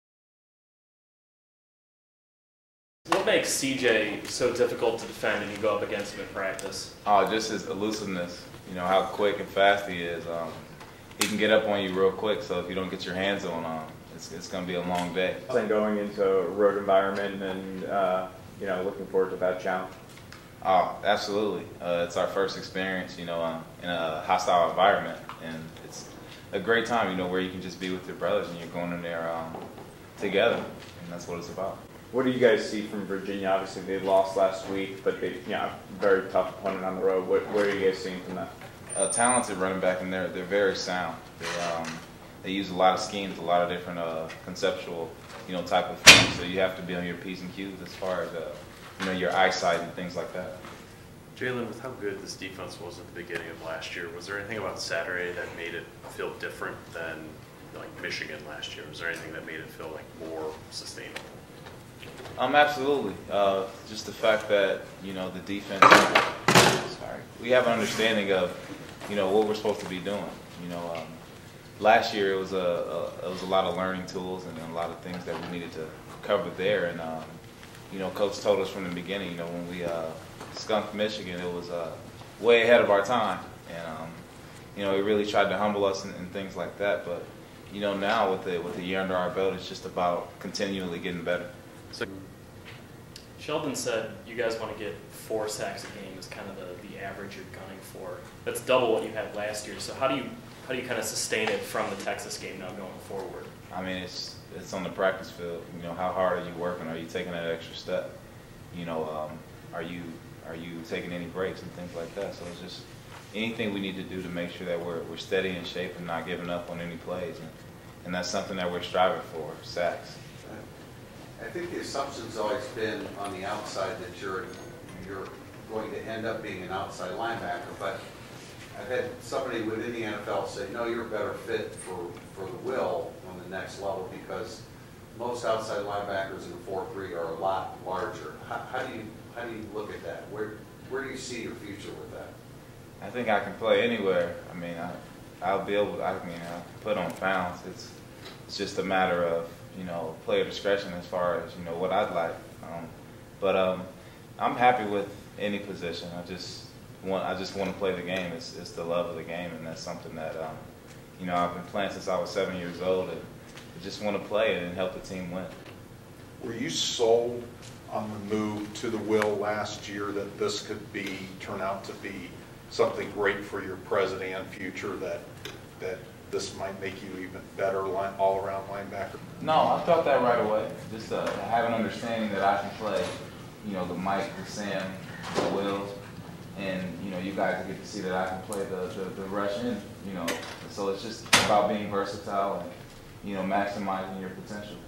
Jaylon Smith - September 9, 2015
This weekend brings the first hostile environment Notre Dame will play in this year with the game against Virginia.